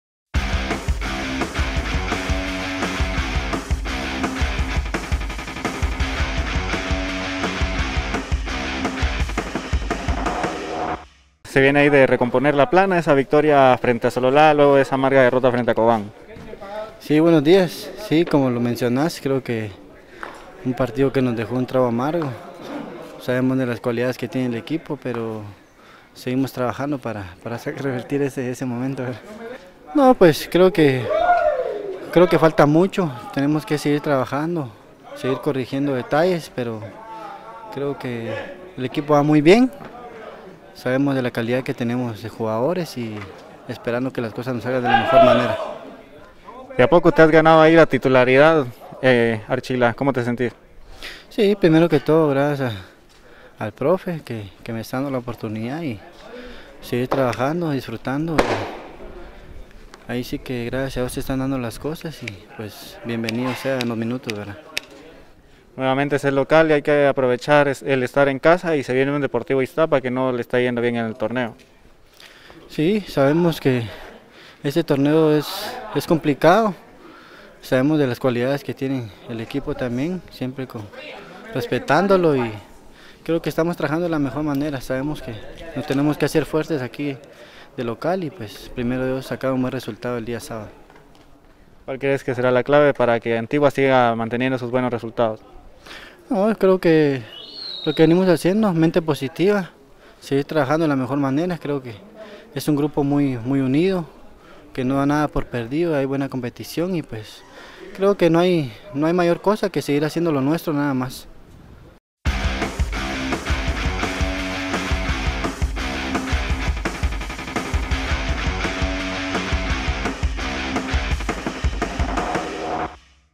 Archila “Un grupo muy unido que no da nada por perdido”
El volante nacional César Archila del Antigua GFC habla sobre su titularidad con el cuadro colonial, también habla sobre el encuentro ante Iztapa y lo que deberá seguir haciendo el equipo para continuar con los buenos resultados